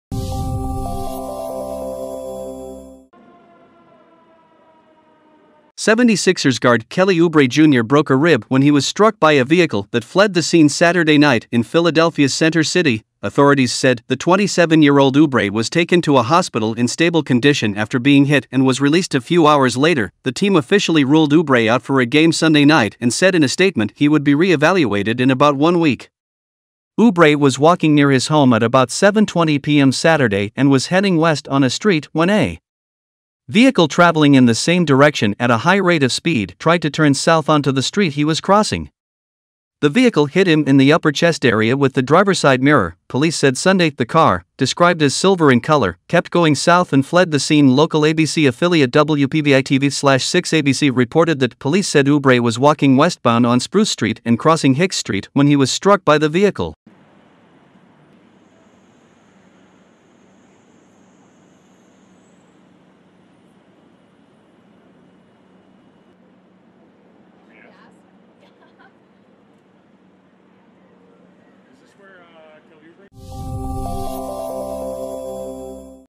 76ers' Kelly Oubre Jr. injured in hit-and-run
76ers guard Kelly Oubre Jr. broke a rib when he was struck by a vehicle that fled the scene Saturday night in Philadelphia's Center City, authorities said.

The 27-year-old Oubre was taken to a hospital in stable condition after being hit and was released a few hours later.

The team officially ruled Oubre out for a game Sunday night and said in a statement he would be re-evaluated in about one week.

Oubre was walking near his home at about 7:20 p.m. Saturday and was heading west on a street when a vehicle travelling in the same direction “at a high rate of speed” tried to turn south onto the street he was crossing. The vehicle hit him “in the upper chest area with the driver-side mirror,” police said Sunday.

The car, described as silver in color, kept going south and fled the scene.

Fire department medics responded and took Oubre to Thomas Jefferson University Hospital “in stable condition with a broken rib and injuries to his hip and right leg,” police said, adding that an investigation is ongoing.